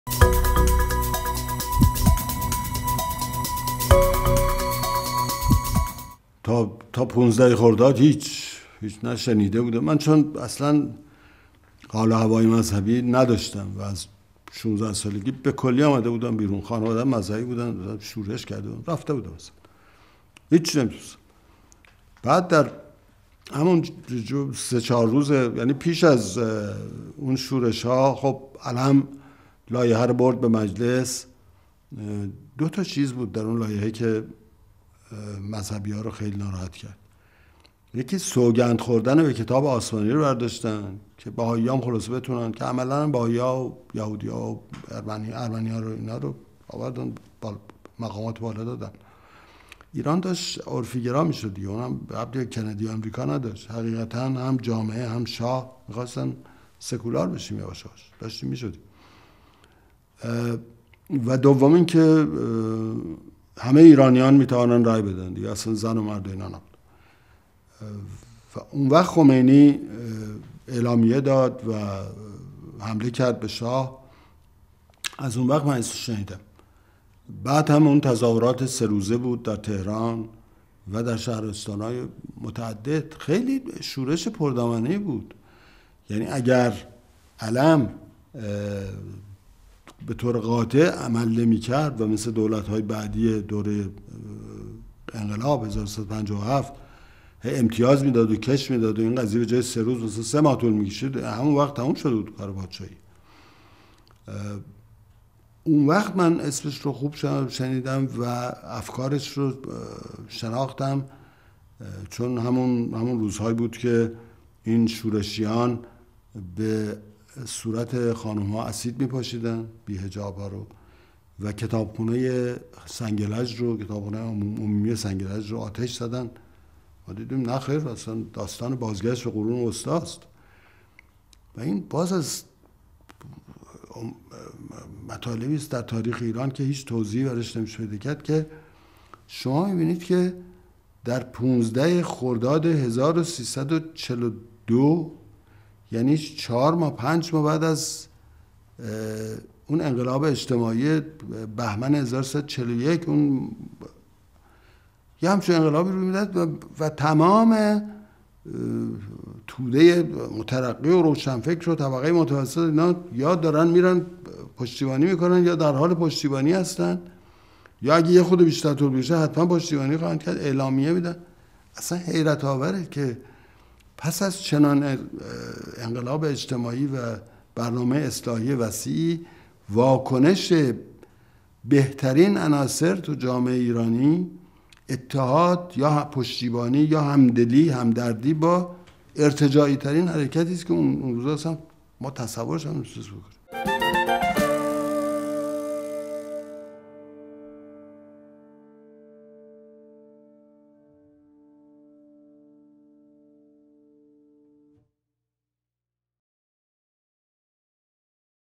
داریوش همایون : نقش خمینی و ۱۵ خرداد در انقلاب
داریوش_همایون روزنامه‌نگار و وزیر اطلاعات و جهانگردی کابینه#آموزگار که زمانی قائم مقام حزب_رستاخیز هم بود می گوید تا ۱۵ خرداد ۱۳۴۲ نام خمینی را نشنیده بود و ۱۵ خرداد در حقیقت قیام علیه حق رای زنان و سوگند نخوردن به کتاب مقدس بود. آن روزها به صورت زنان اسید می‌پاشیدند و کتابخانه سنگلچ را آتش زدند. بیشتر گروه‌های سیاسی و روشنفکران هم از قیام مرتجعین مذهبی حمایت کردند.
«روایت‌های انقلاب»، سری گفت‌و‌گوهای مازیار بهاری با انقلابیون از
گرایش‌های مختلف سیاسی است که در سال ۱۳۸۵ و به منظور ثبت تاریخ شفاهی انقلاب_اسلامی ۱۳۵۷ انجام شده و به مناسبت چهل_سالگی_انقلاب منتشر می‌شود.